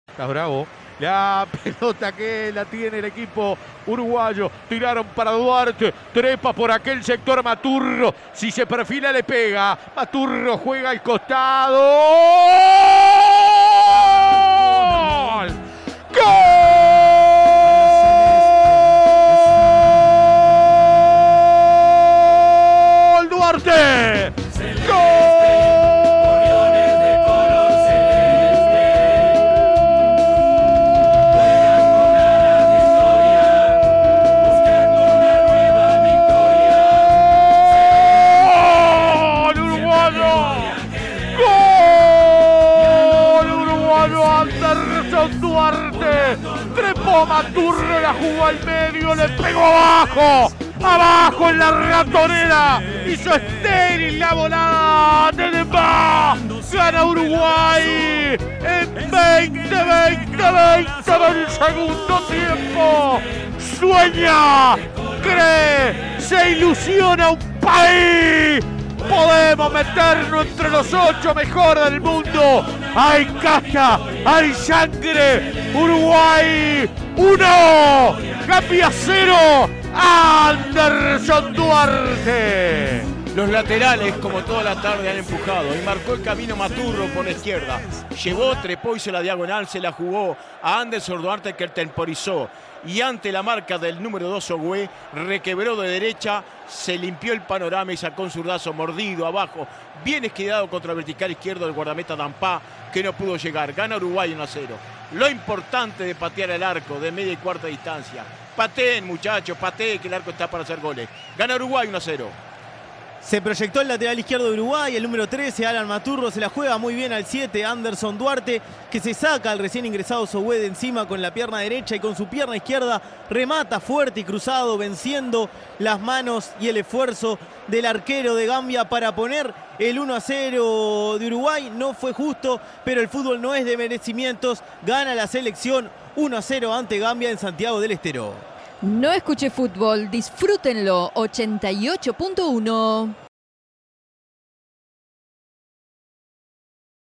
Uruguay 1 Gambia 0 - Copa del Mundo Sub-20 2023 (por Luis Morales)
El gol del partido, por Luis Morales junto a Leonardo Amarilla y Franco Abreu para 88.1 FM Punta del Este.